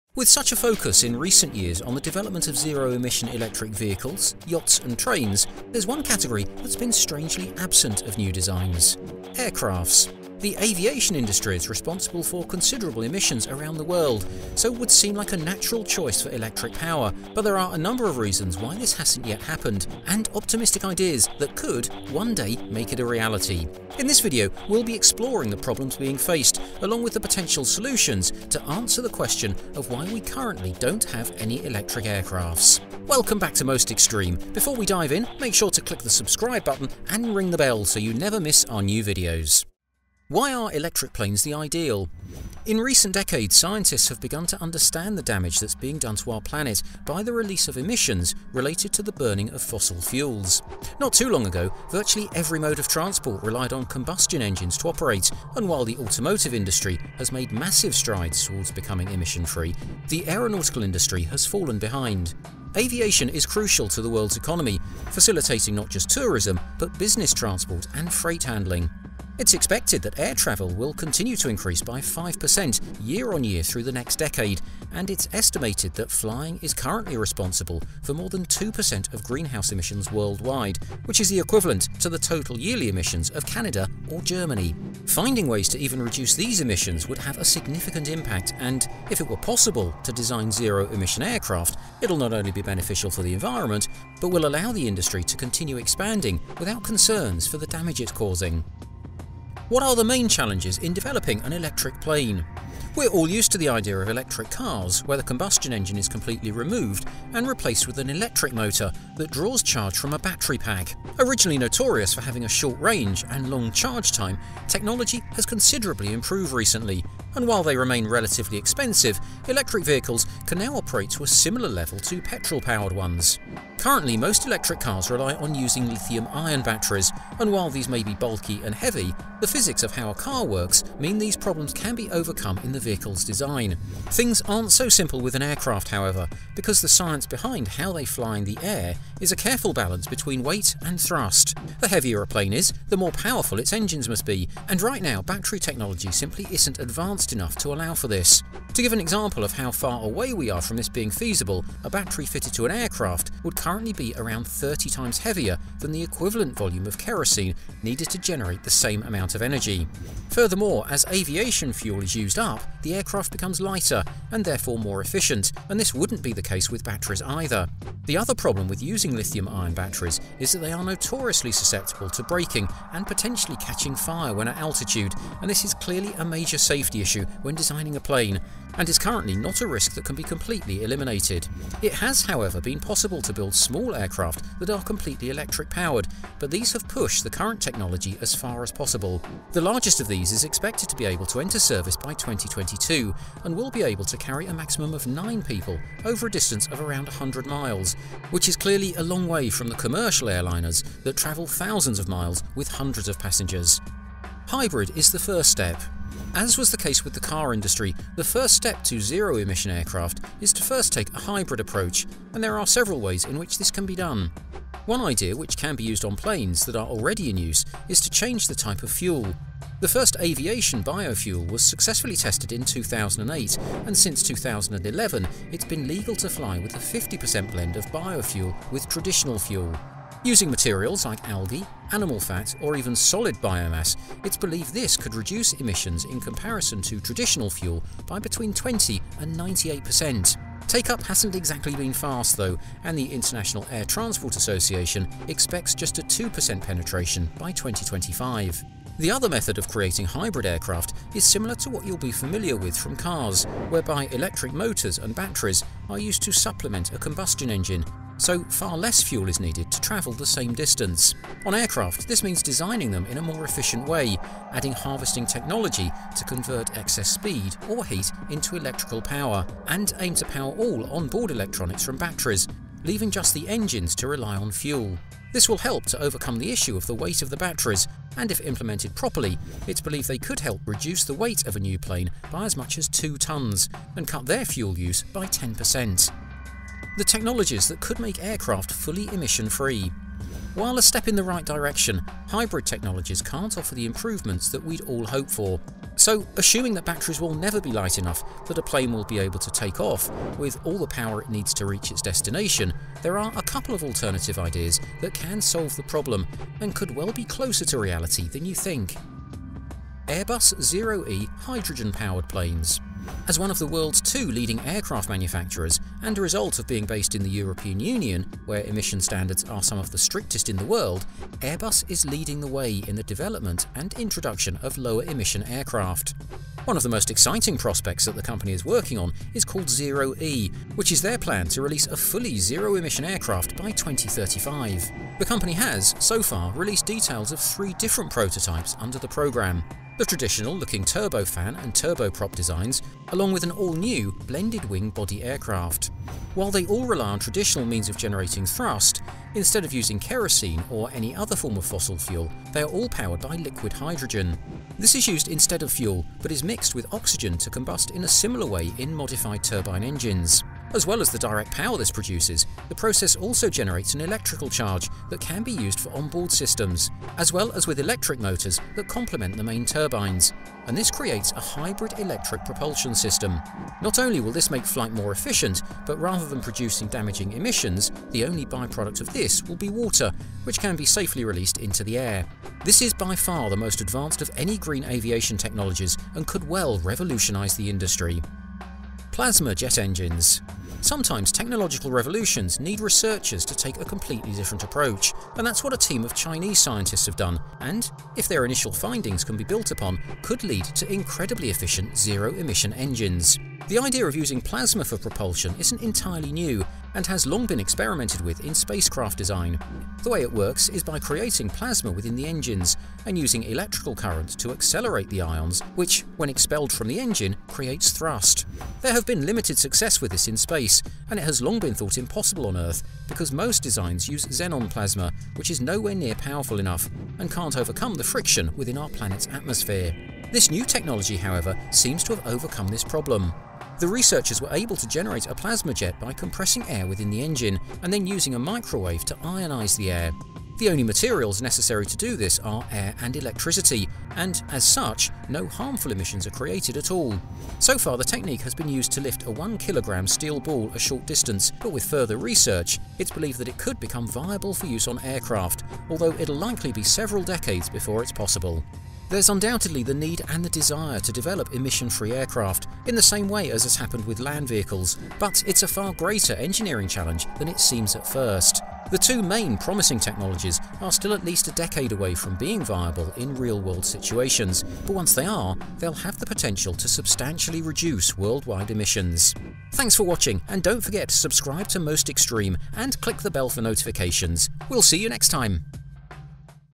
Are Electric Planes Possible? Hydrogen & Plasma Technologies
With such a focus in recent years on the development of zero-emission electric vehicles, yachts, and trains, there's one category that's been strangely absent of new designs… aircrafts. The aviation industry is responsible for considerable emissions around the world, so would seem like a natural choice for electric power, but there are a number of reasons why this hasn't yet happened, and optimistic ideas that could, one day, make it a reality. In this video we'll be exploring the problems being faced, along with the potential solutions, to answer the question of why we currently don't have electric aircraft.

00:00 Intro
00:40 Why Are Electric Planes the Ideal?
01:39 What are the Main Challenges in Developing an Electric Plane?
03:24 Hybrid is the First Step
04:46 The Technologies That Could Make Aircraft Fully Emission Free
05:10 AirBus ZeroE Hydrogen Powered Planes
06:29 Plasma Jet Engines